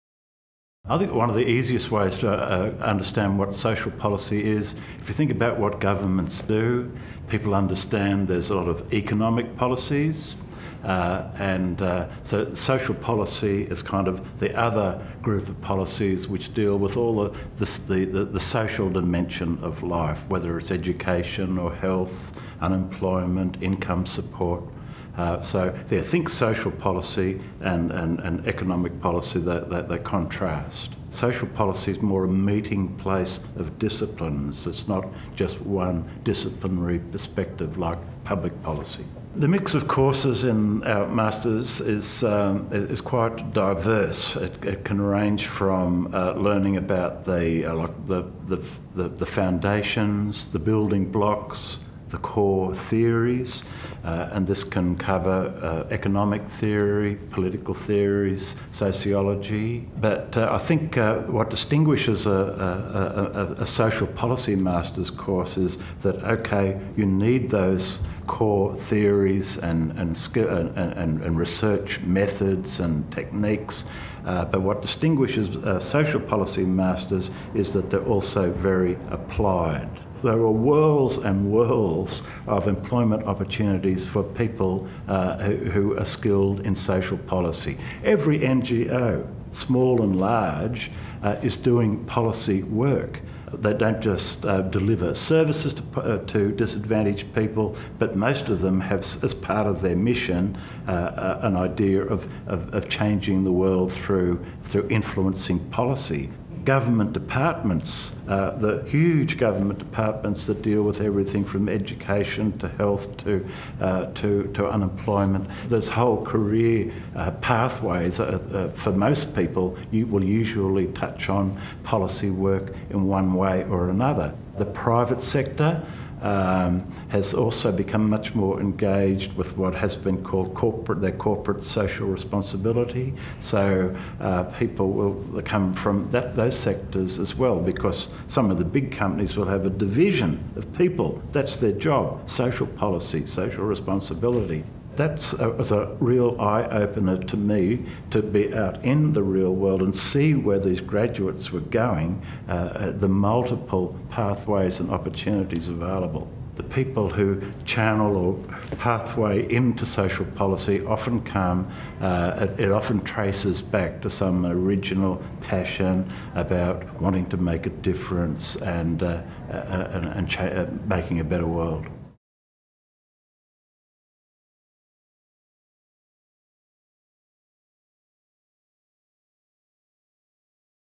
Master of Social Policy course co-ordinator, Professor Paul Smyth
What is social policy? Professor Paul Smyth, course co-ordinator of the Master of Social Policy at the University of Melbourne, explains.